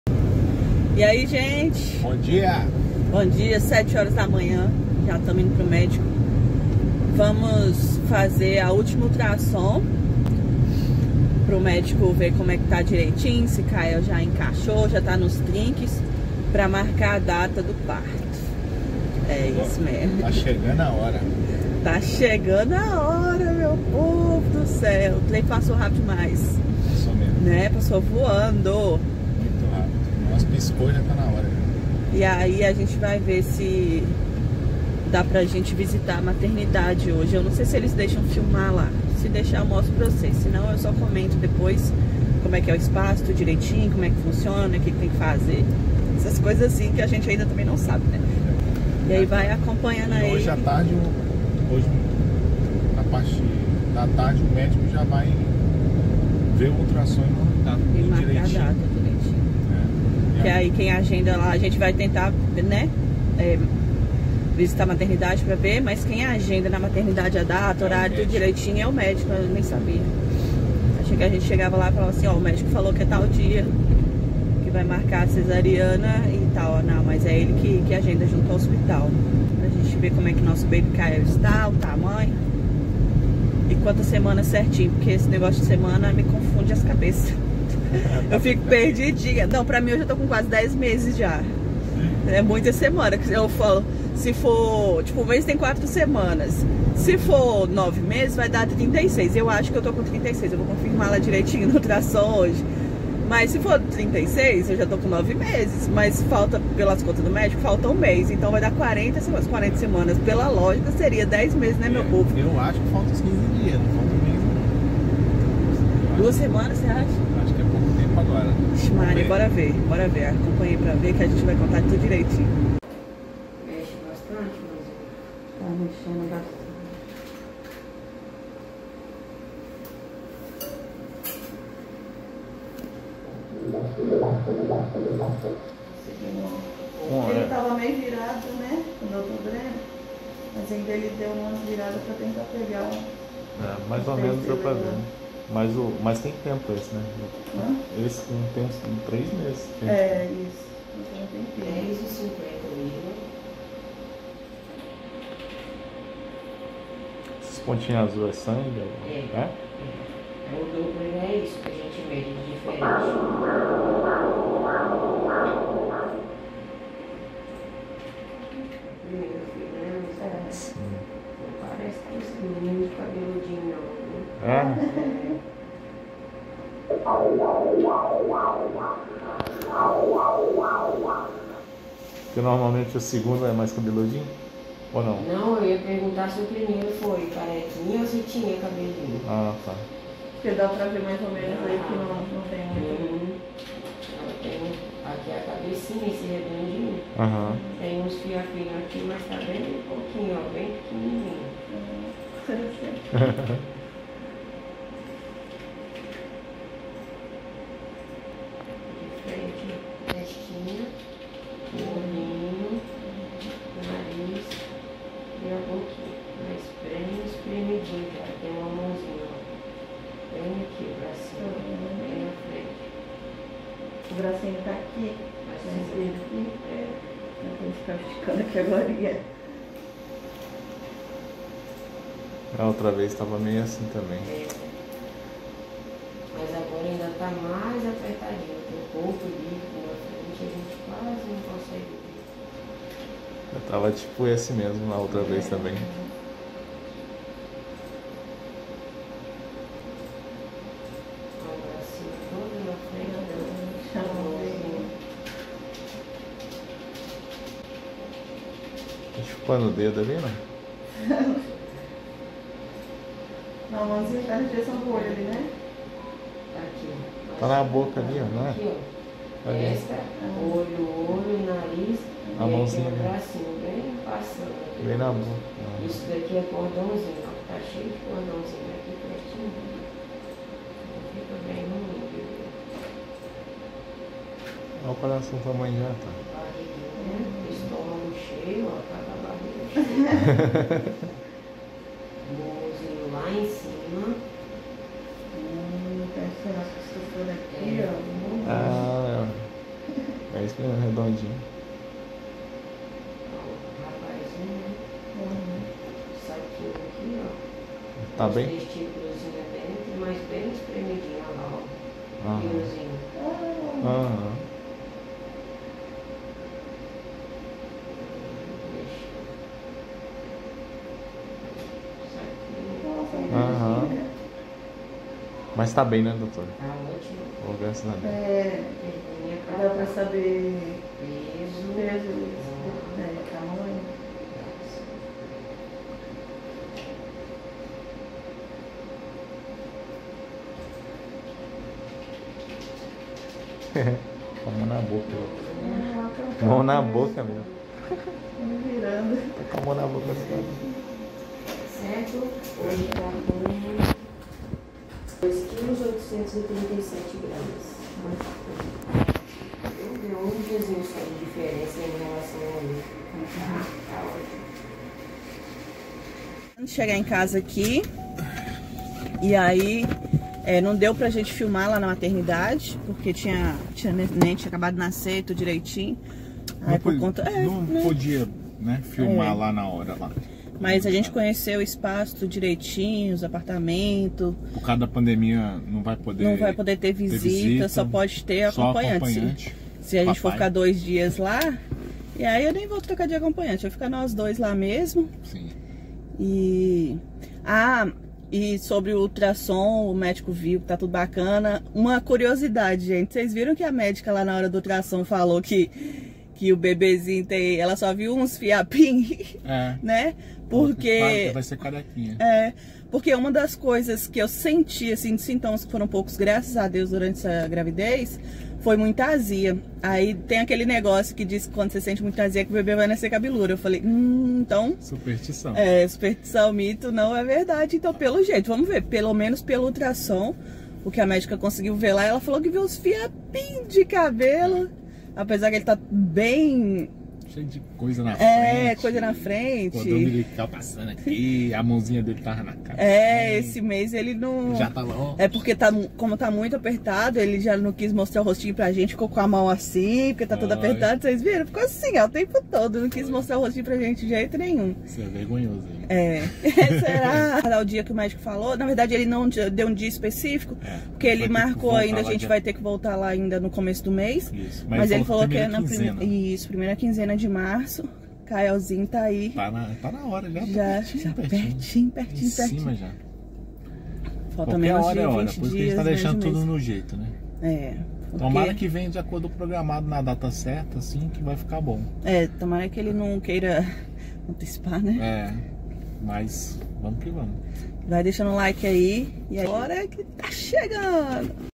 ÚLTIMO ULTRASSOM DOPPLER ANTES DO KAEL NASCER - ESTÁ CHEGANDO A HORA
Hoje vamos fazer o último ultrasson, esse é com doppler que é pra ver toda circulação sanguínia, vasos e muito mais, agora é só aguardar a chegada do nosso príncipe Kael 💙👶.
Ele só não quis tirar a mãozinha do rosto pois já está faltando espaço dentro do forninho, está se sentindo apertado, louco pra sair 🤭🤣🤣